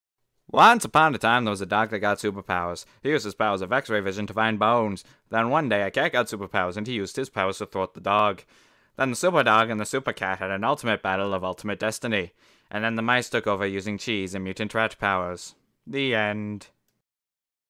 Superdog vs. Supercat
A touching story of romance.
Yes, I plan on making these short, I really just planned for these to be something small and short I could do that didn't involve a whole lot of work because, well, I'm lazy.